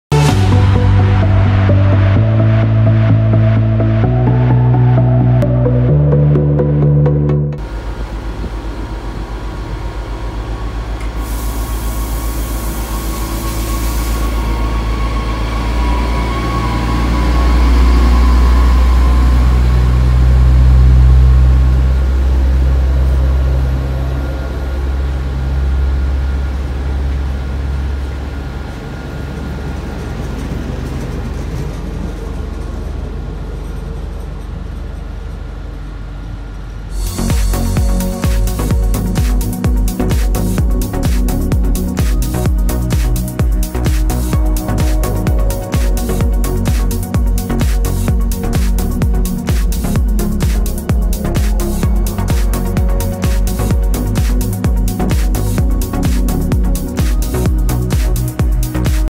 [Toulouse-Matabiau] Départ AGC B 81500 Bombardier - livrée bleu/gris (ancienne livrée Midi-Pyrénées)
Départ AGC B 81500 Bombardier - livrée bleu/gris (ancienne livrée Midi-Pyrénées) - mode thermique
Rame n°81808 - destination Capdenac